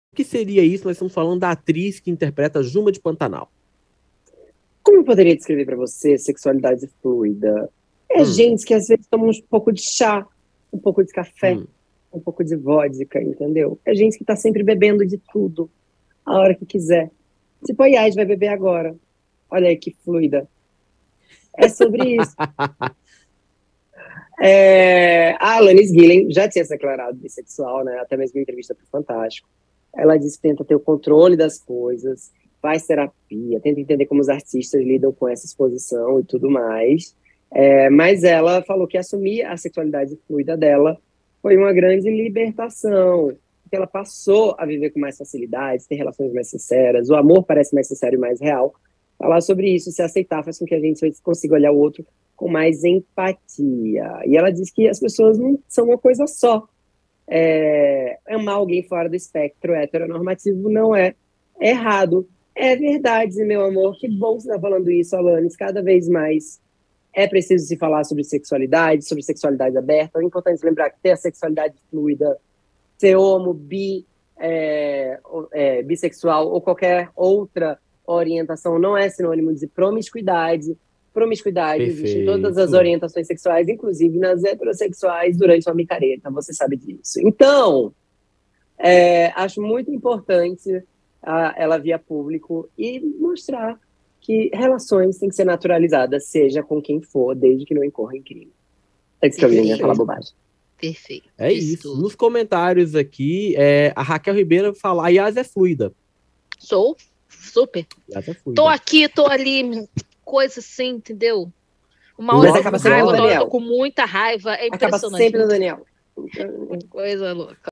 ALANIS GUILLEN: Atriz que faz JUMA em PANTANAL assume sexualidade FLUIDA
Protagonista de "Pantanal" (TV Globo), a atriz Alanis Guillen encontra-se no melhor momento de sua carreira. Tamanha visibilidade, no entanto, também acarreta em alguns desafios - e a jovem de 24 anos ainda luta para conseguir lidar.